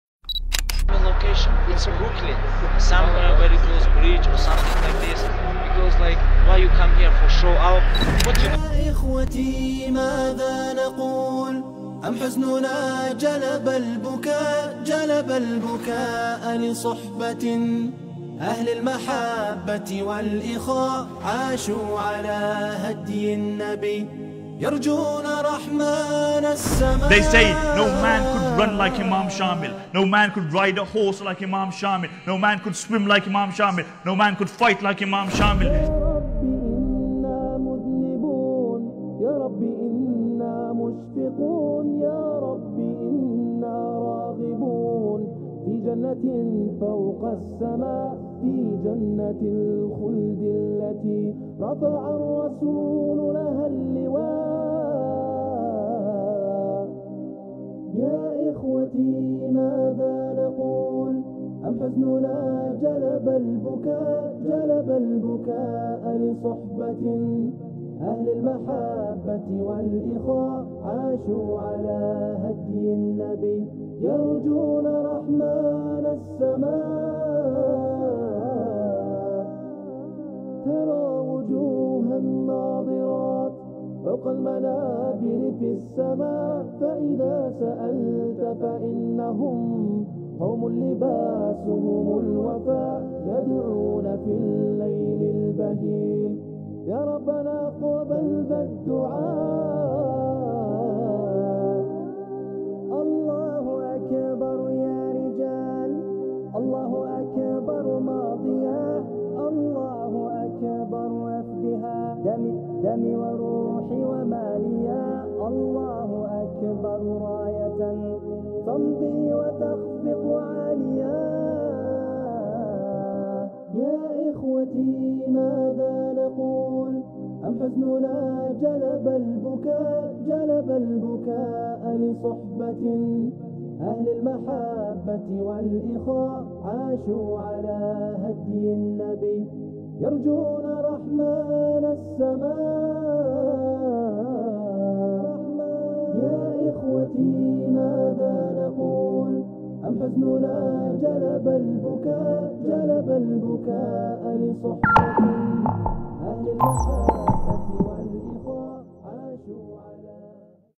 UFC Champion Khabib Background Check
This is a History of Khabib Great Grandfather And where his Lineage has Come From its Fair to Say this Man Is A Warrior Because its in His Blood.